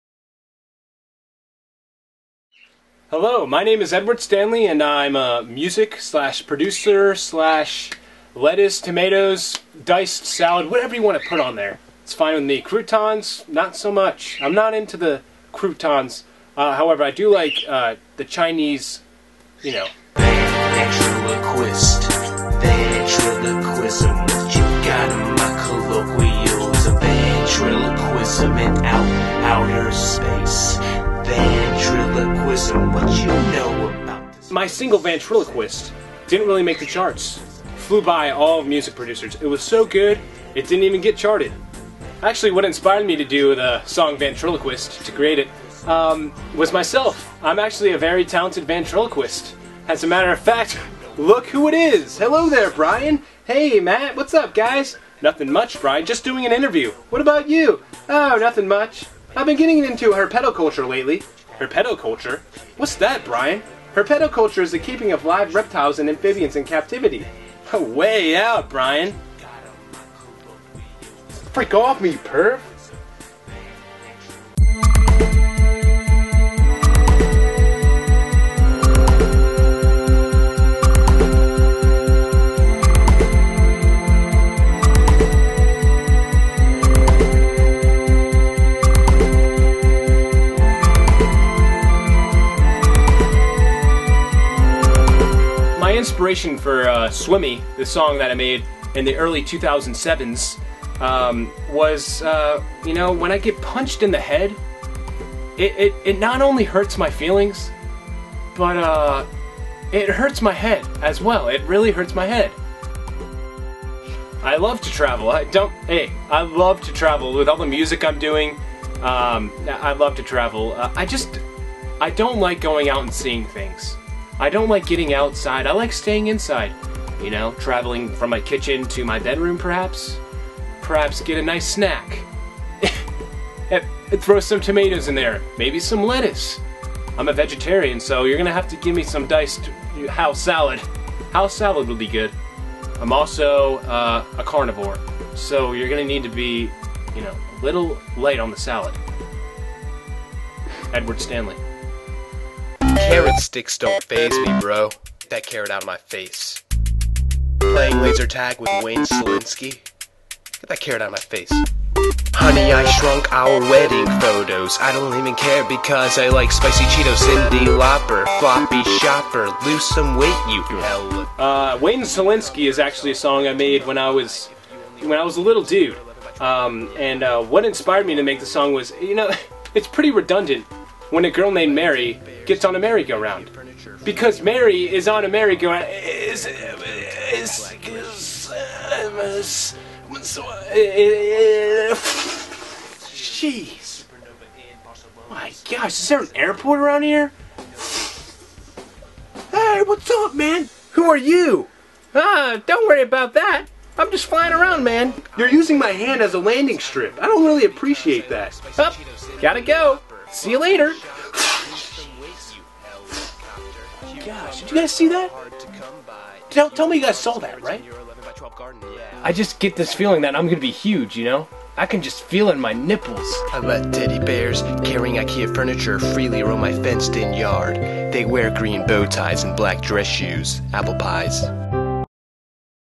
Edward Stanley
This is a Documentary about the Music/Producer/Salad Edward Stanley

Shot and Edited by:Don Greger
Edward Stanley: Matthew DeHoff
Featuring: Justin Greger